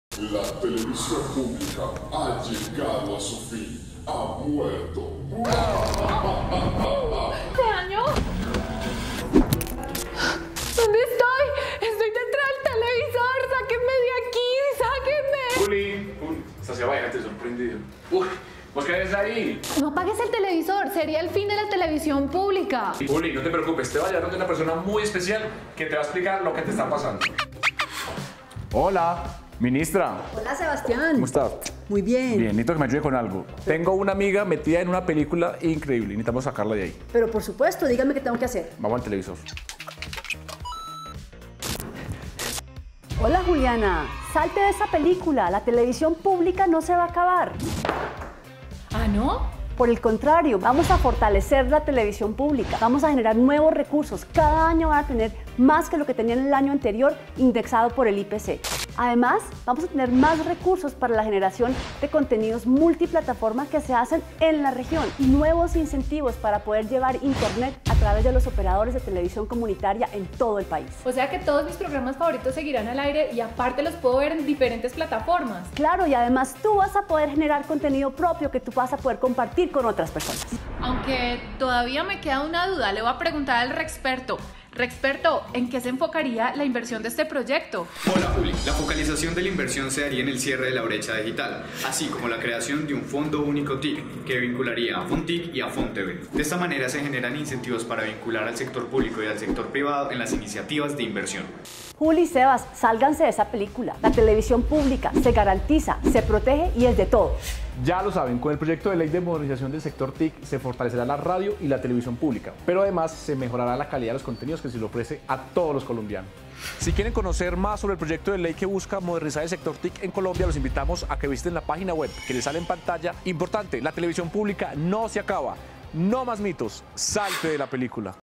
Salte de esa película, la televisión pública no desaparece. C41 N2
Ahora la televisión pública se fortalece gracias al proyecto de Ley para Modernizar el sector TIC.